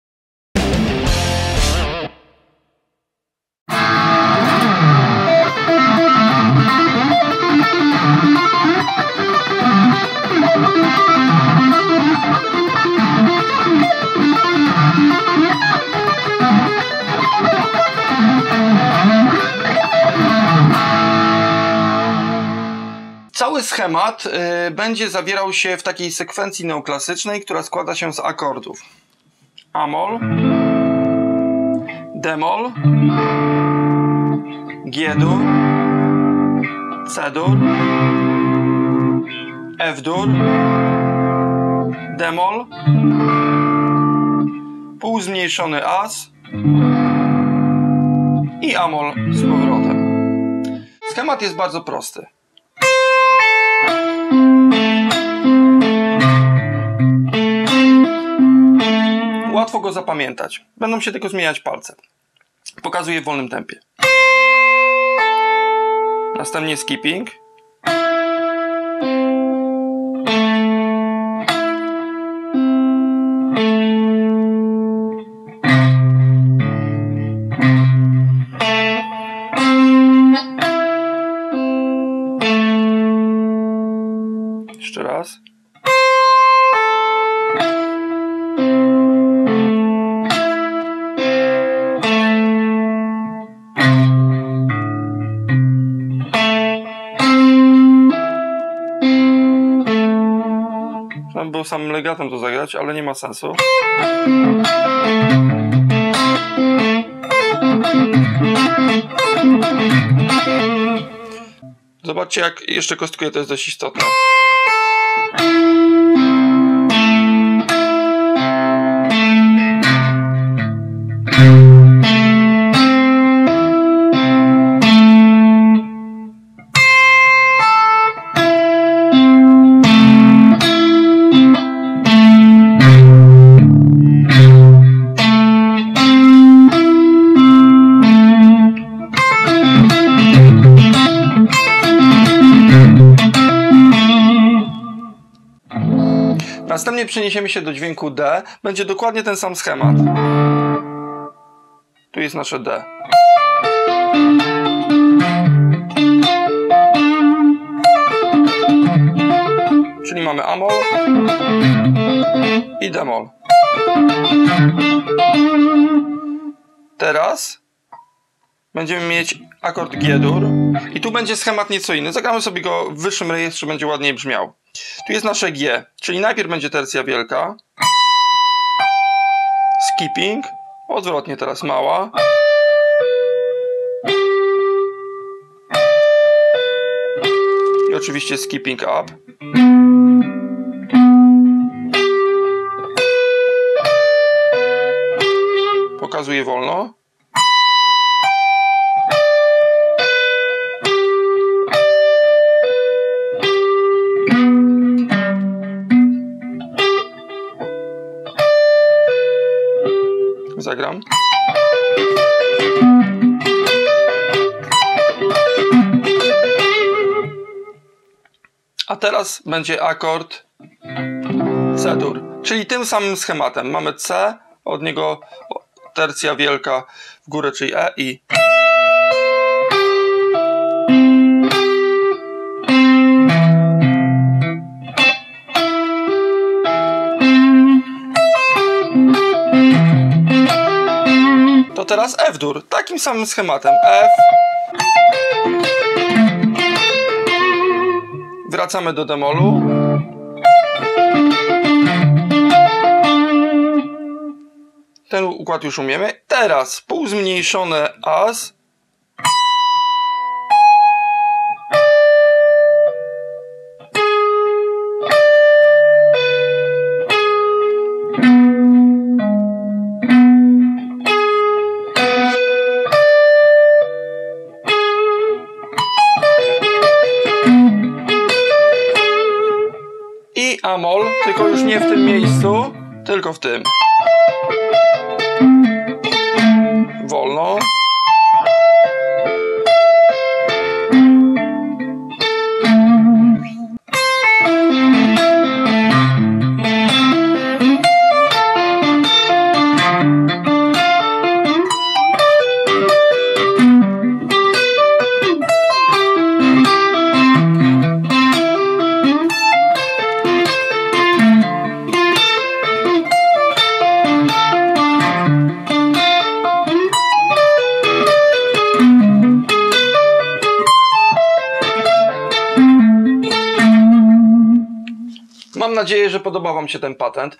"Shred Not Dead" pt. 1 - Skipping | Guitar Center PL
Zapraszamy do ćwiczenia trudniejszych tematów, takich jak skipping połączony z legatami. Temat zabarwiony neoklasycyzmem, lecz naprawdę wymagający! Ogień z gryfu!!!